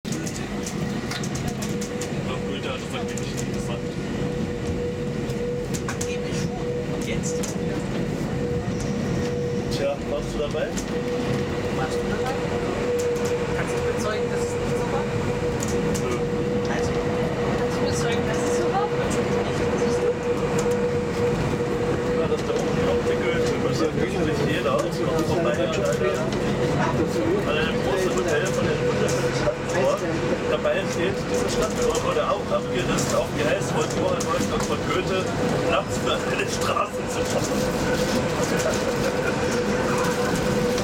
Fahrt mit der Zahnradbahn auf die Zugspitze: Tunnelfahrt
Eine kurze Fahrtstrecke mit der Zugspitz-Zahnradbahn im Tunnel!